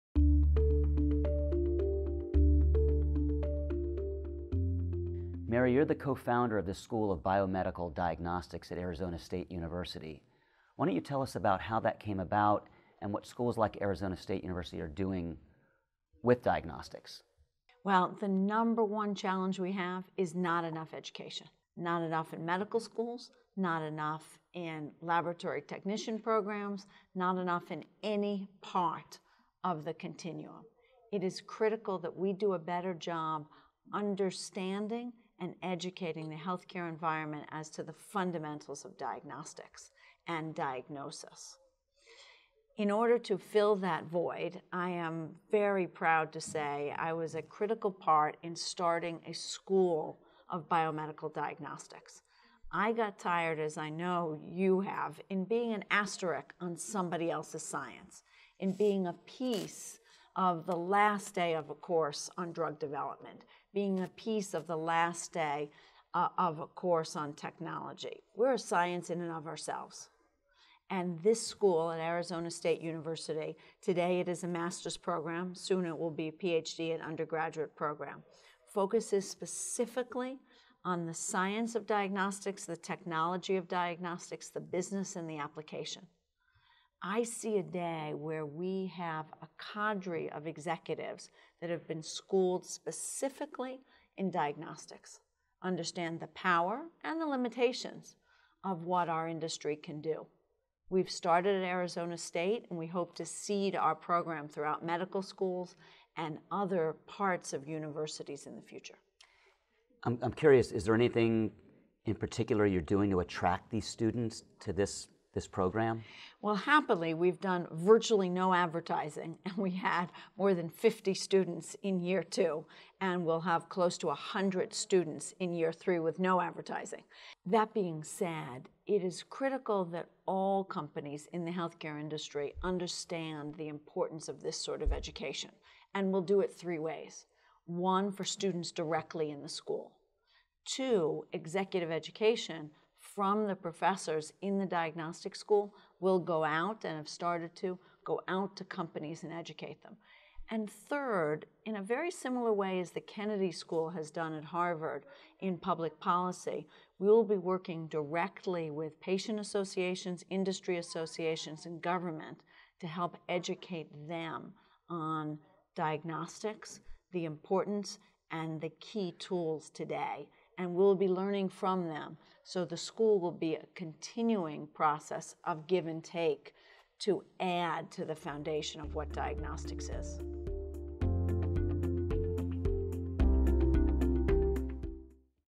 Mara G. Aspinall - School of Biomedical Diagnostics at Arizona State University
Adam Slone, CEO of Slone Partners, sits down with healthcare industry pioneer Mara G. Aspinall in Boston, MA to discuss hiring and retaining the best people before an acquisition, the International School of Biomedical Diagnostics at Arizona State University, and her vision for the next great shift in healthcare, Diagnostics 5.0.